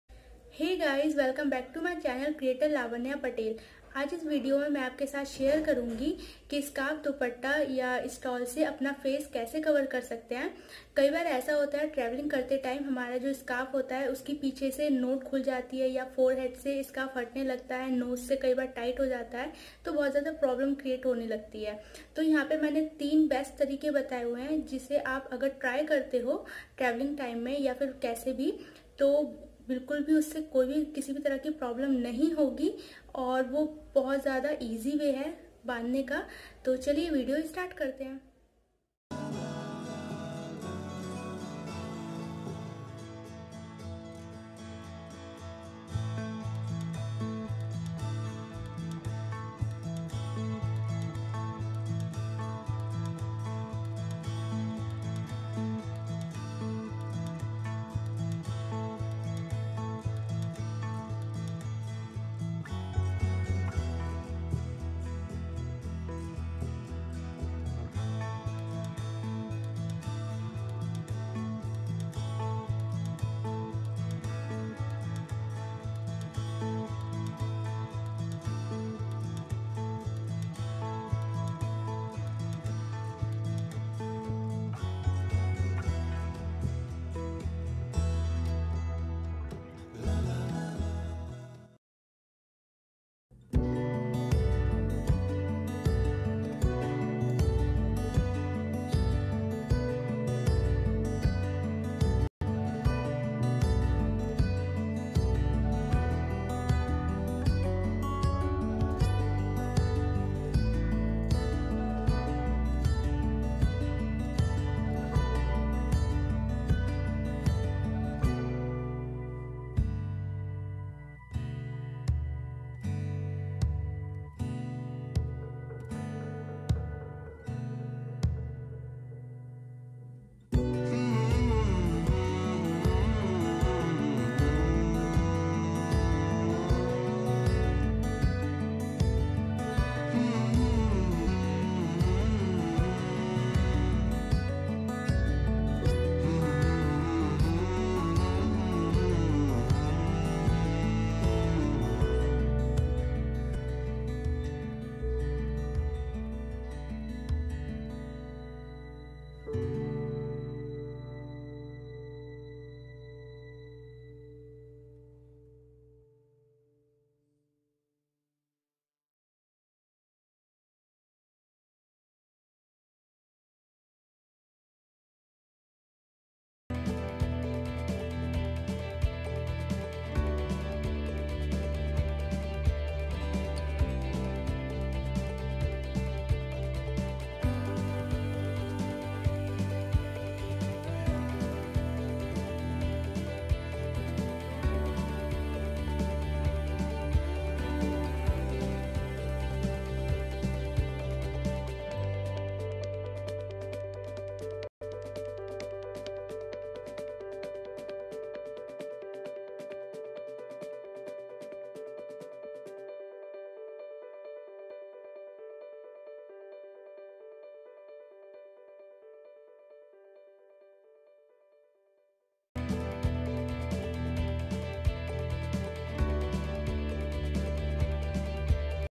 How to wrap face with Scarf or Dupatta || easy way to wrap scarf || 2020
Hello everyone !!

Today's video is about how to cover your face with Scarf. In these days it is necessary to cover your face due to corona virus which is very harmful for everyone therefore this video will helpful to cover your face.